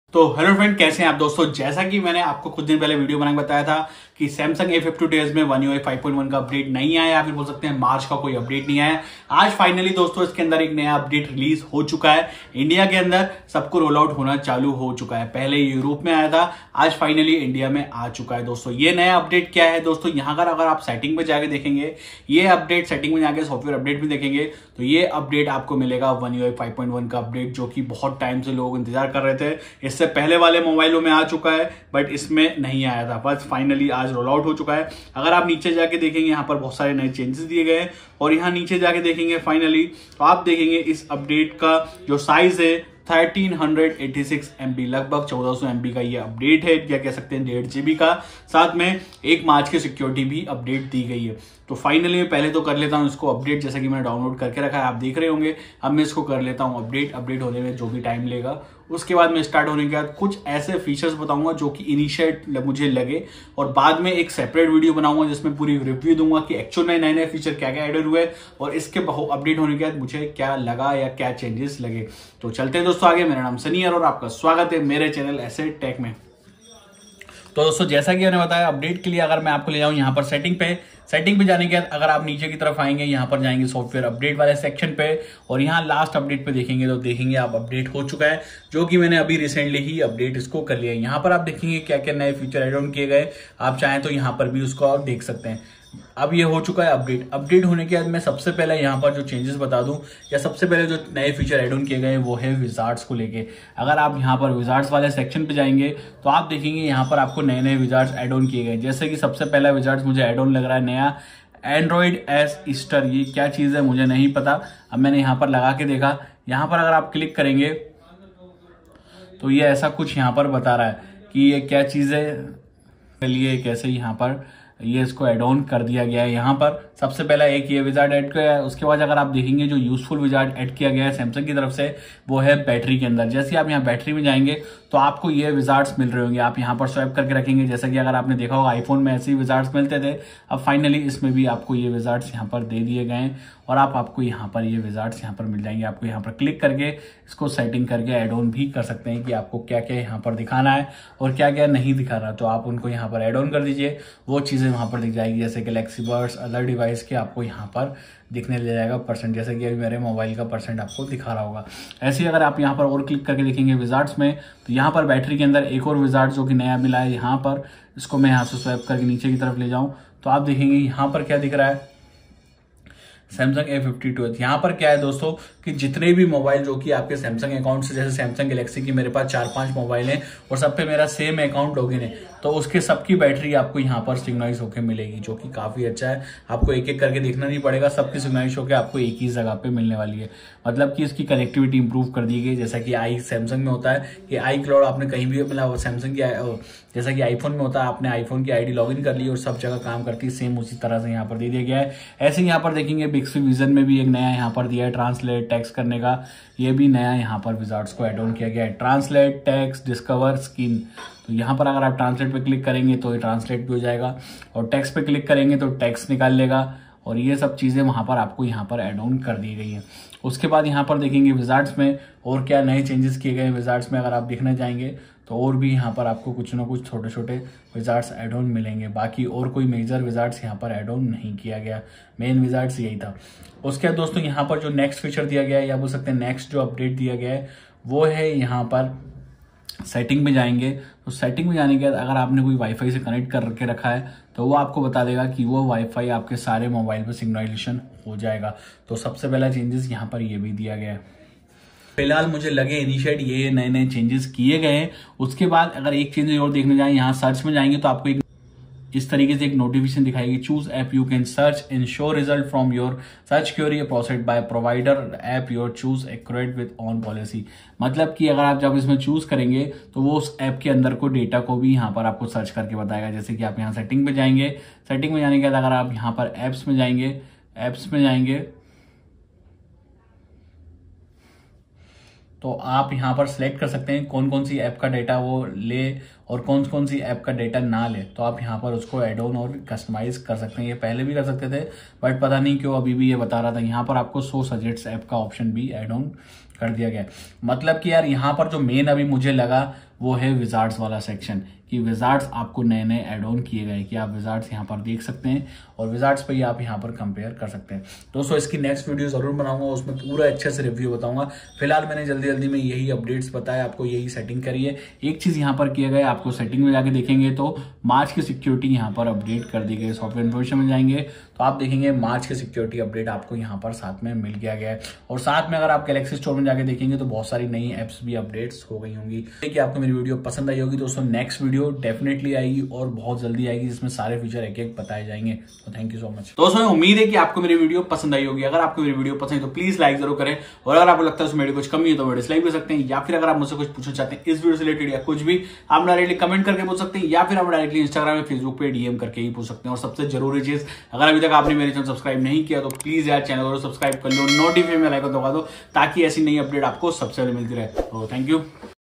Finally Samsung A52s One Ui 5.1 Update Released
In this video i describe about Samsung a52s one ui 5.1 new update and features March 2023